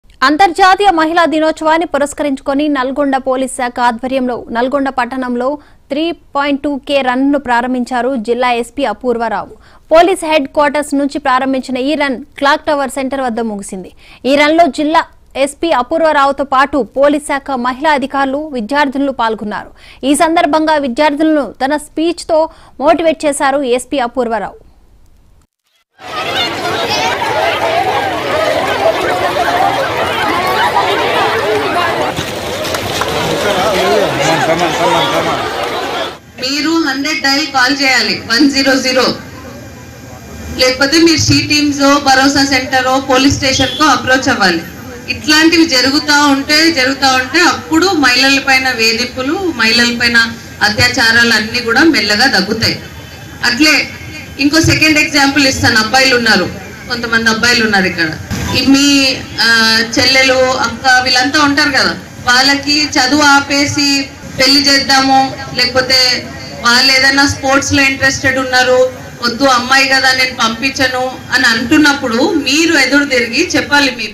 నల్గొండ పోలీస్ శాఖ ఆధ్వర్యంలో 3.2K రన్ | Nalgonda District | SP Apoorva Rao | Mahaa News
నల్గొండ పోలీస్ శాఖ ఆధ్వర్యంలో 3.2K రన్
1PM